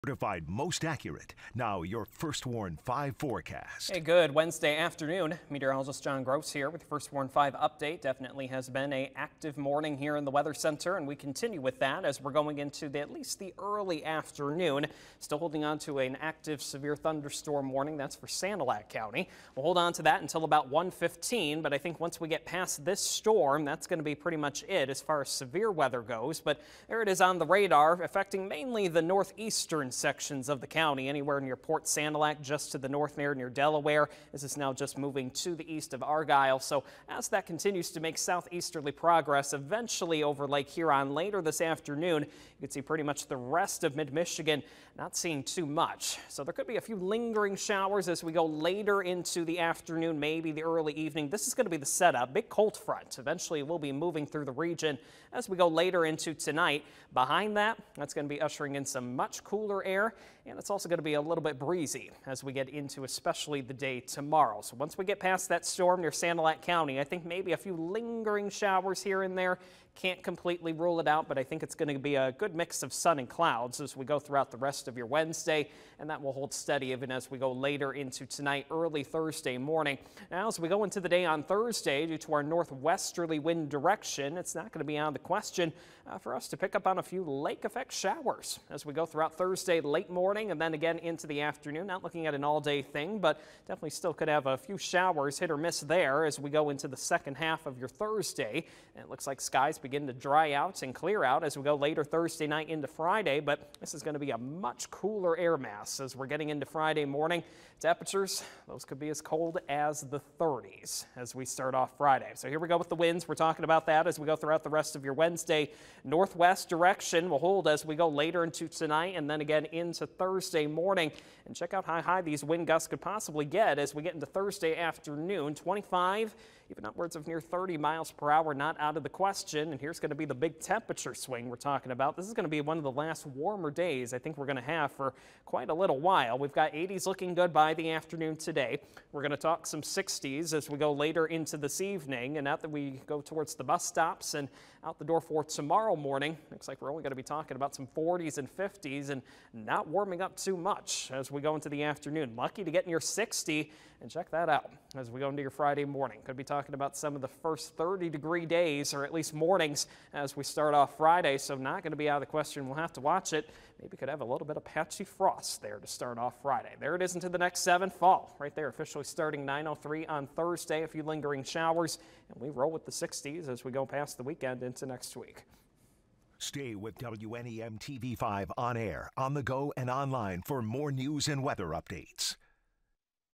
First Warn 5: Wednesday afternoon, Sept. 21
Meteorologist John Gross has your First Warn 5 Forecast.